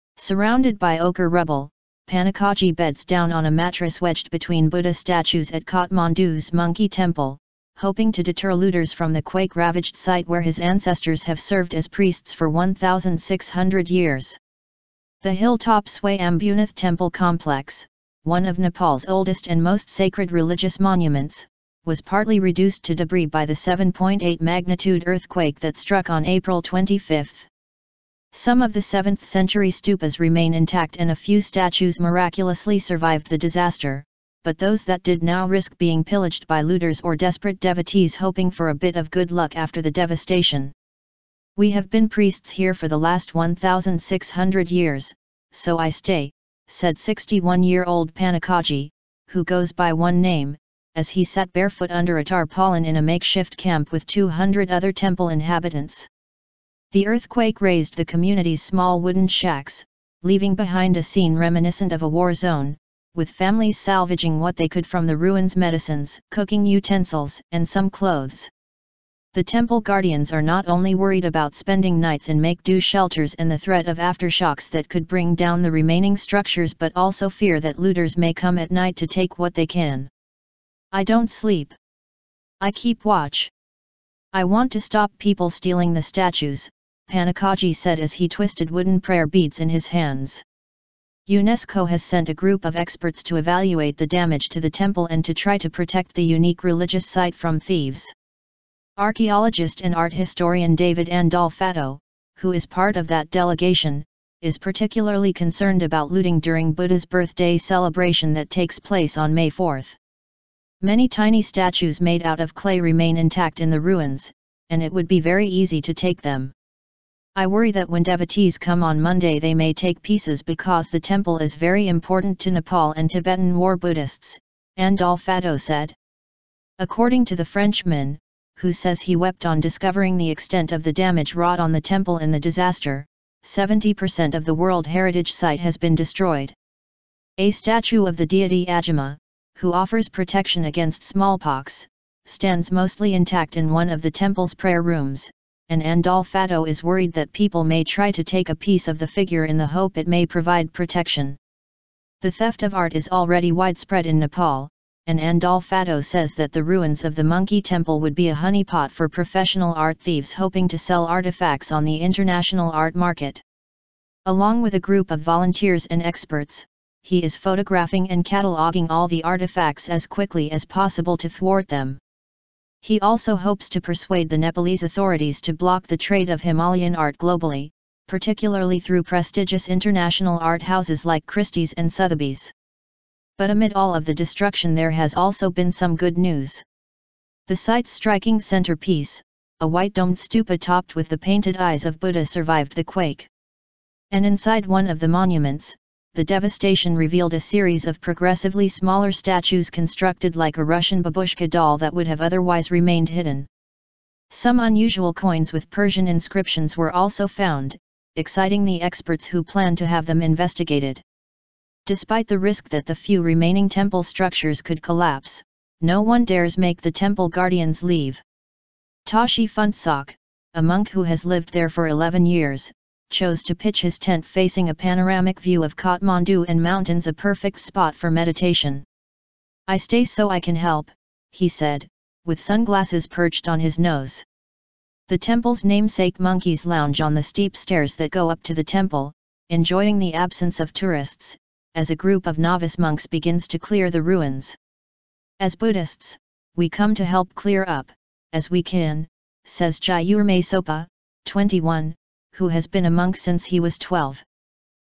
Nepal quake-ravaged temple faces threat from looters
Surrounded by ochre rubble, Pannakaji beds down on a mattress wedged between Buddha statues at Kathmandu's "Monkey Temple", hoping to deter looters from the quake-ravaged site where his ancestors have served as priests for 1,600 years.

The hilltop Swayambunath Temple complex, one of Nepal's oldest and most sacred religious monuments, was partly reduced to debris by the 7.8 magnitude earthquake that struck on April 25.

Some of the seventh-century stupas remain intact and a few statues miraculously survived the disaster, but those that did now risk being pillaged by looters or desperate devotees hoping for a bit of good luck after the devastation.

"We have been priests here for the last 1,600 years, so I stay," said 61-year-old Pannakaji, who goes by one name, as he sat barefoot under a tarpaulin in a makeshift camp with 200 other temple inhabitants.

The earthquake razed the community's small wooden shacks, leaving behind a scene reminiscent of a war zone, with families salvaging what they could from the ruins -- medicines, cooking utensils and some clothes.

The temple guardians are not only worried about spending nights in make-do shelters and the threat of aftershocks that could bring down the remaining structures -- but also fear that looters may come at night to take what they can.

"I don't sleep. I keep watch. I want to stop people stealing the statues," Pannakaji said as he twisted wooden prayer beads in his hands.

UNESCO has sent a group of experts to evaluate the damage to the temple and to try to protect the unique religious site from thieves.

Archeologist and art historian David Andolfatto, who is part of that delegation, is particularly concerned about looting during Buddha's birthday celebration that takes place on May 4.

"Many tiny statues made out of clay remain intact in the ruins, and it would be very easy to take them. I worry that when devotees come on Monday they may take pieces because the temple is very important to Nepal and Tibetan Newar Buddhists," Andolfatto said.

According to the Frenchman, who says he wept on discovering the extent of the damage wrought on the temple in the disaster, 70 percent of the World Heritage site has been destroyed.

A statue of the deity Ajima, who offers protection against smallpox, stands mostly intact in one of the temple's prayer rooms, and Andolfatto is worried that people may try to take a piece of the figure in the hope it may provide protection.

The theft of art is already widespread in Nepal, and Andolfatto says that the ruins of the Monkey Temple would be a honeypot for professional art thieves hoping to sell artifacts on the international art market.     Along with a group of volunteers and experts, he is photographing and cataloguing all the artifacts as quickly as possible to thwart them.

He also hopes to persuade the Nepalese authorities to block the trade of Himalayan art globally, particularly through prestigious international art houses like Christie's and Sotheby's.

But amid all of the destruction there has also been some good news.

The site's striking centrepiece, a white-domed stupa topped with the painted eyes of Buddha survived the quake.

And inside one of the monuments, the devastation revealed a series of progressively smaller statues -- constructed like a Russian babushka doll -- that would have otherwise remained hidden.

Some unusual coins with Persian inscriptions were also found, exciting the experts who plan to have them investigated.

Despite the risk that the few remaining temple structures could collapse, no one dares make the temple guardians leave.

Tashi Phuntsok, a monk who has lived there for 11 years, chose to pitch his tent facing a panoramic view of Kathmandu and mountains -- a perfect spot for meditation.

"I stay so I can help," he said, with sunglasses perched on his nose.

The temple's namesake monkeys lounge on the steep stairs that go up to the temple, enjoying the absence of tourists, as a group of novice monks begins to clear the ruins.

"As Buddhists, we come to help clear up, as we can," says Gyurmey Sopa, 21, who has been a monk since he was 12.